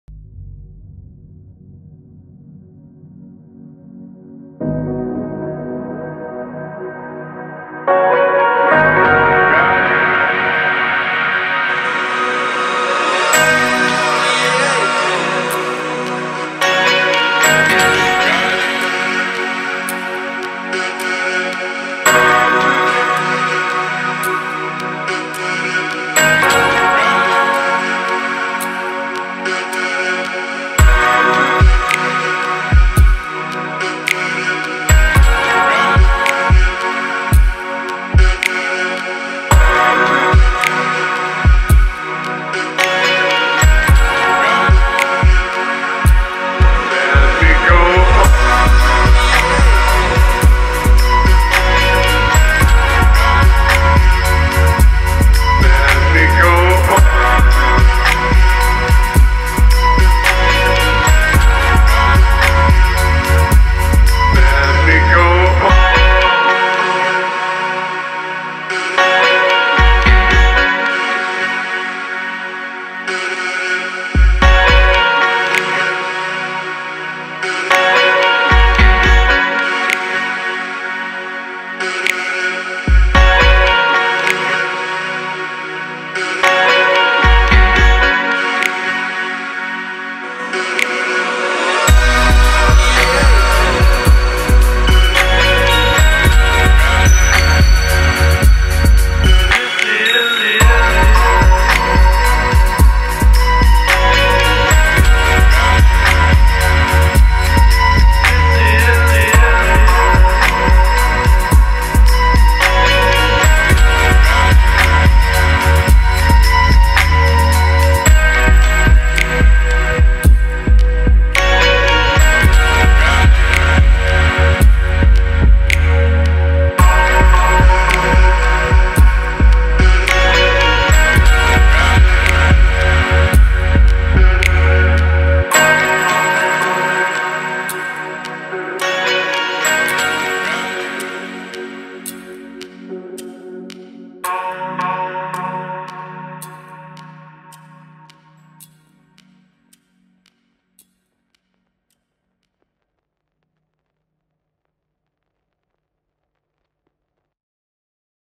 GHz Media: 4K Colorado Fall
Shot by our Inspire 1 quadcopter in various Colorado places. Enjoy!